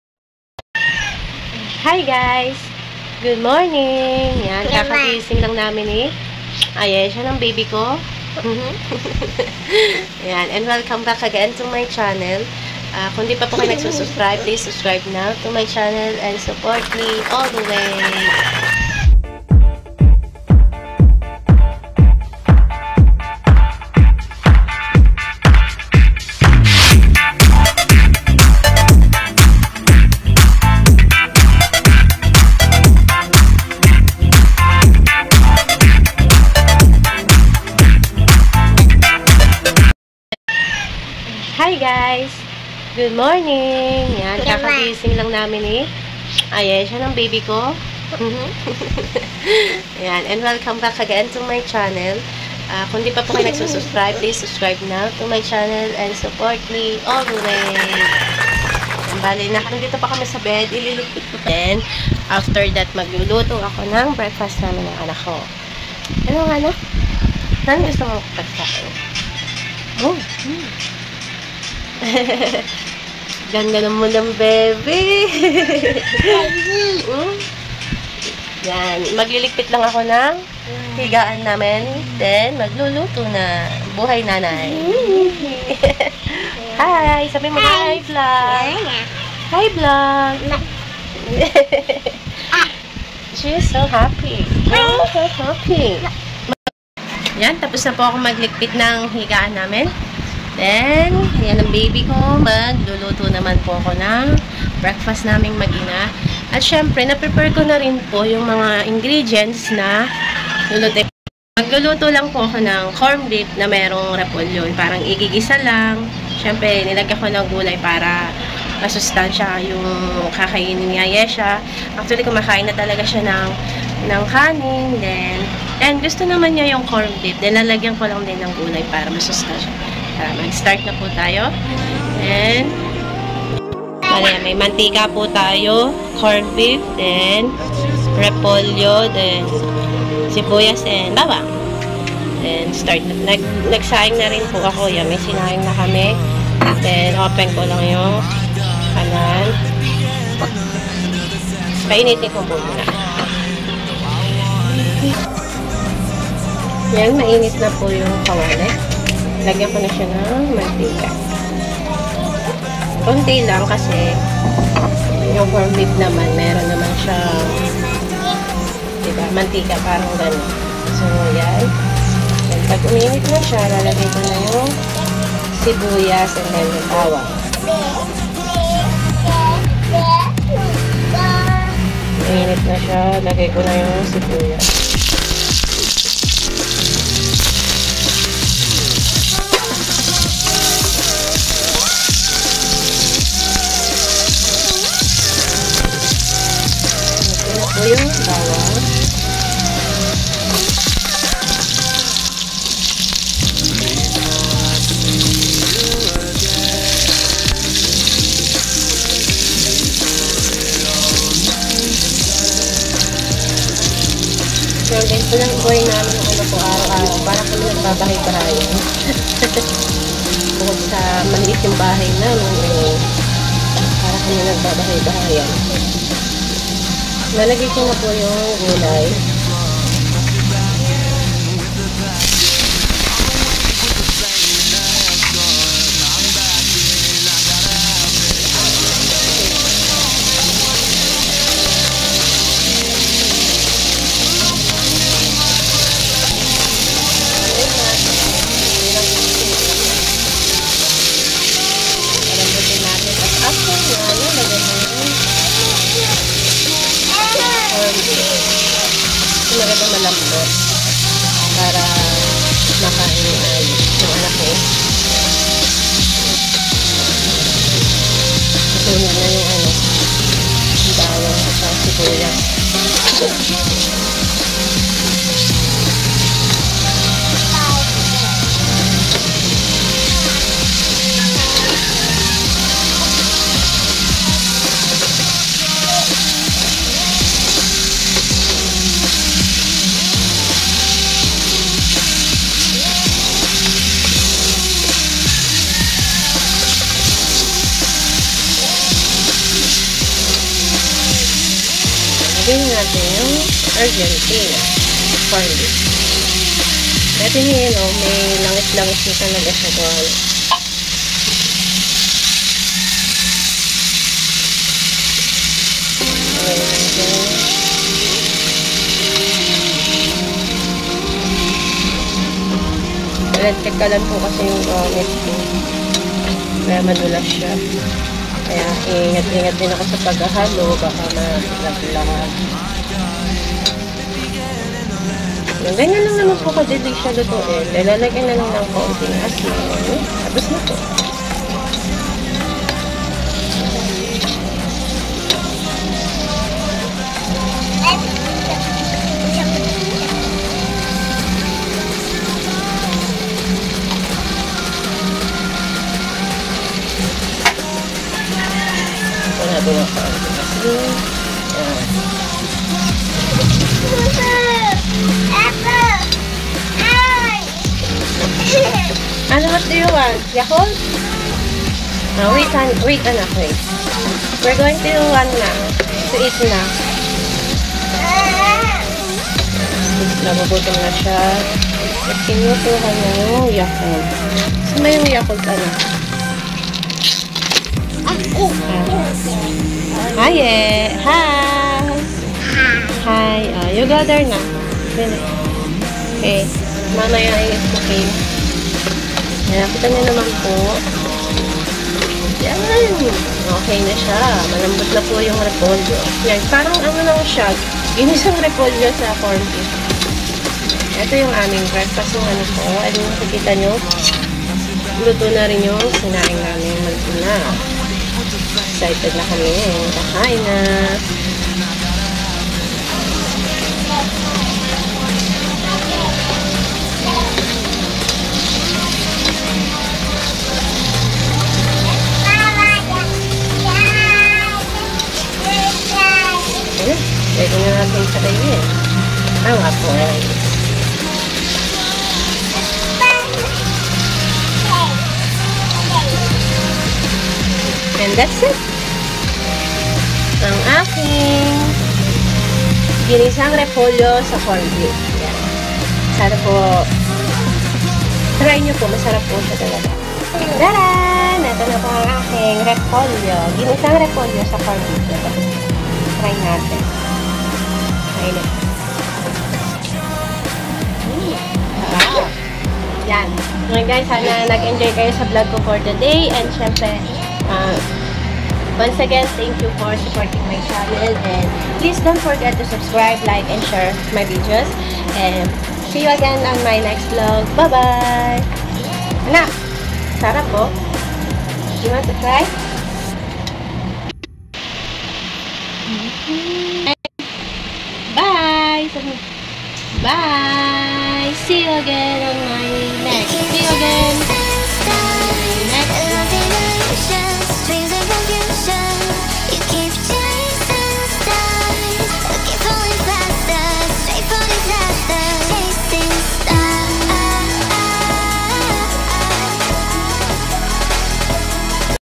How To Cook Cornbeef with Veggies
Hello EveryJuan now I'm going to cook Sauteed El Cabbage con Cornbeef for our breakfast. Hope u like my video for today..

Sauteed El Cabbage con Cornbeef
Ingredients:
-Oil
-Cornbeef
-Cabbage
-Onion
-Garlic
-Salt

Mary Sundy Leopando

msleopando34

sundy261